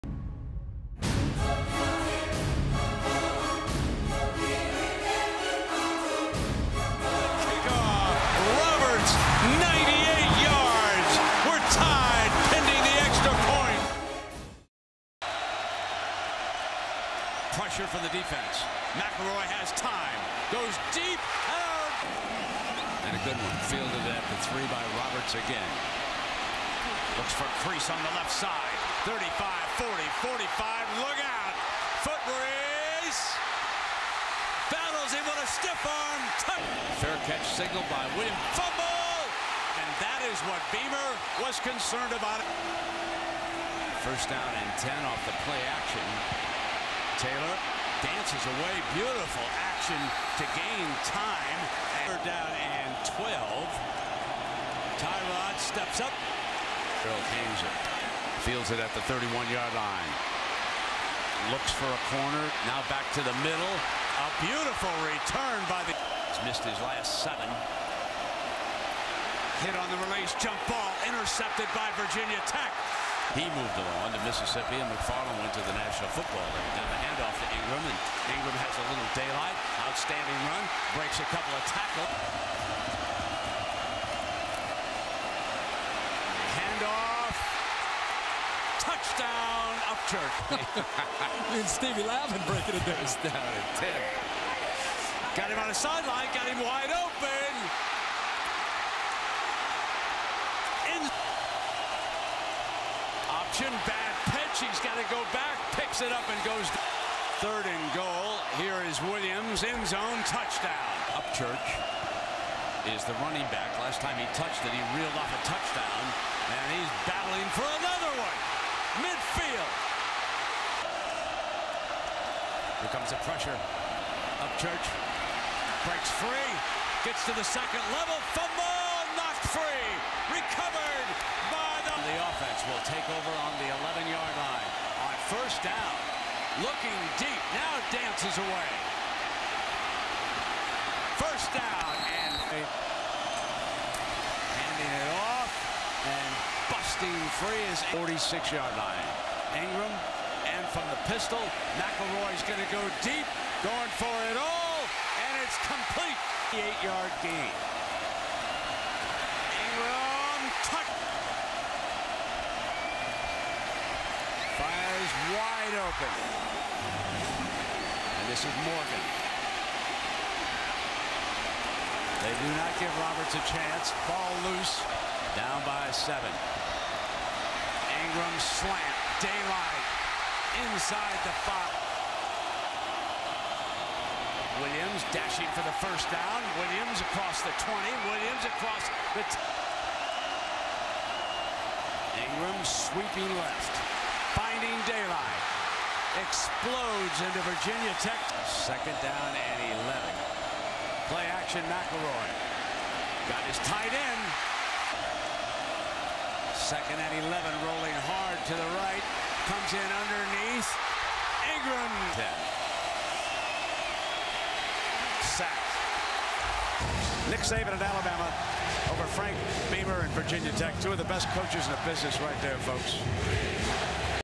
2009 Virginia Tech Hokies vs Alabama Crimson Tide Highlights (HD)
Highlights from the 2009 Virginia Tech vs Alabama football game.

Copyright: ESPN/ABC SPORTS